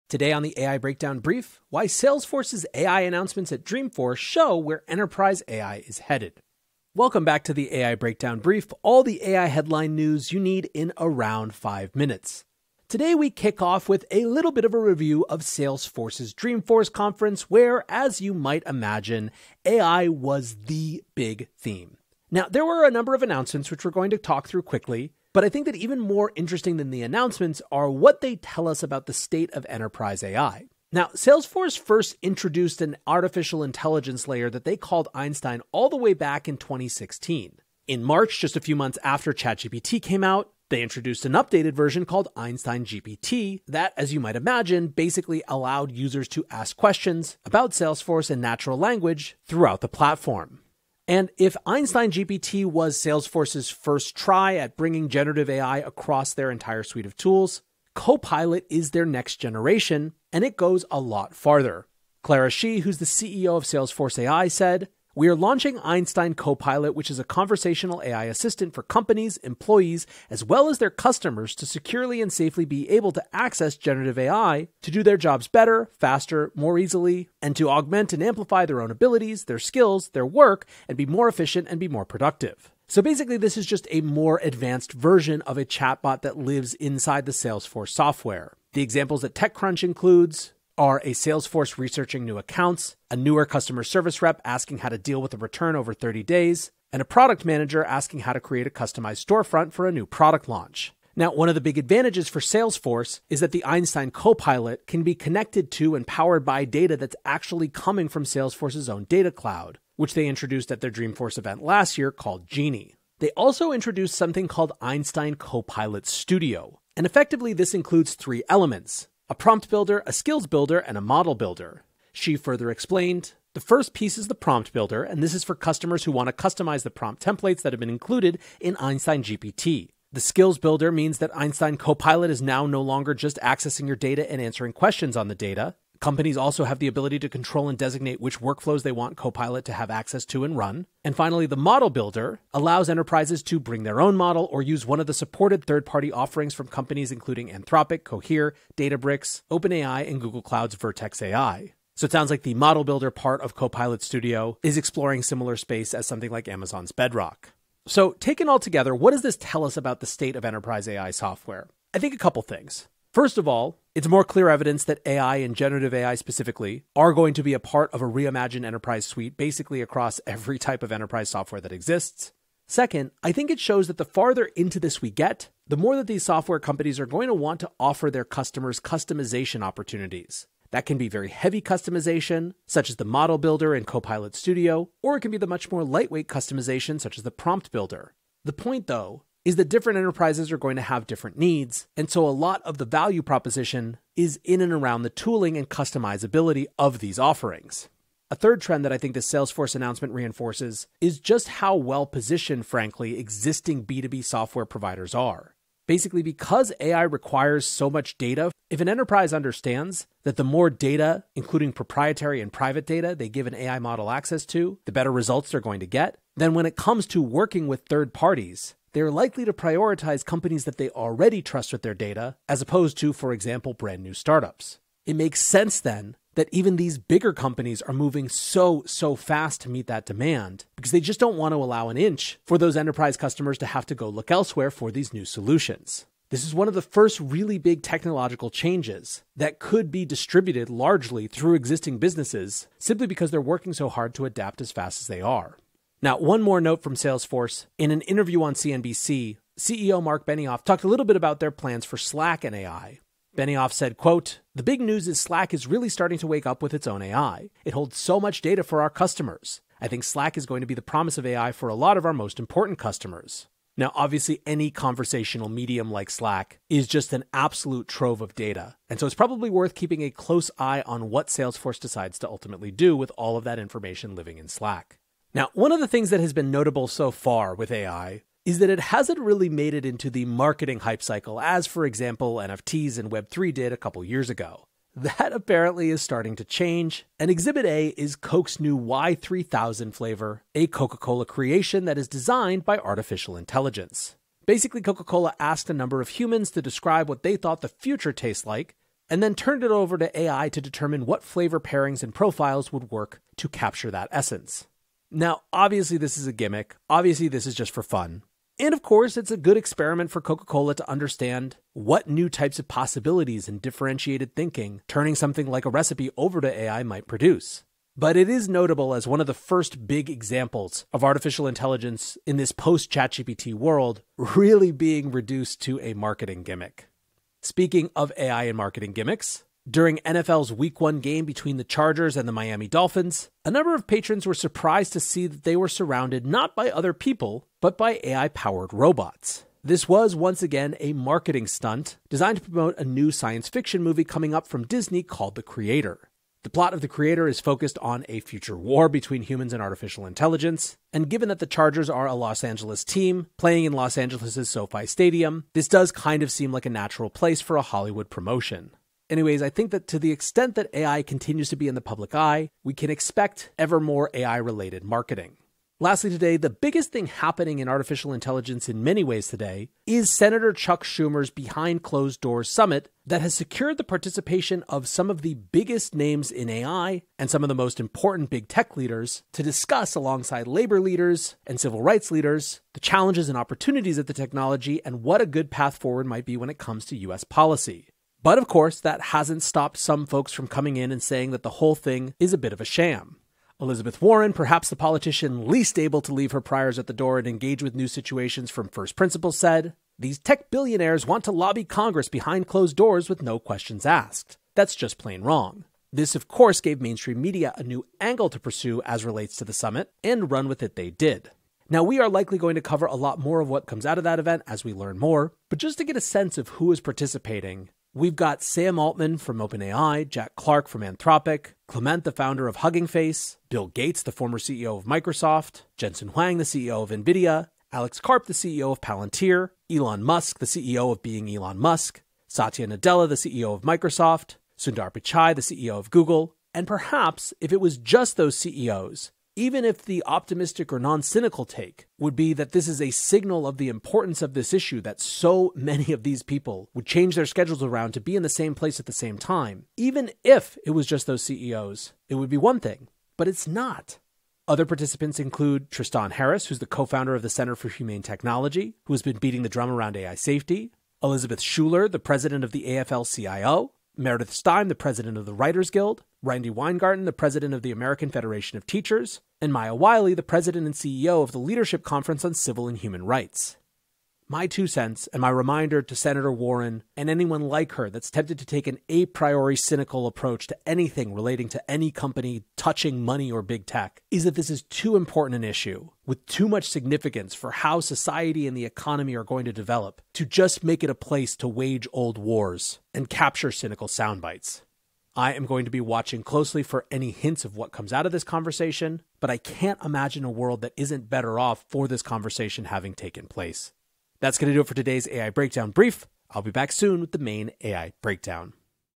The Future of Enterprise AI is Customizability
At the Dreamforce conference in San Francisco this week we got a preview not only of their new AI features, but of the future of enterprise AI in general.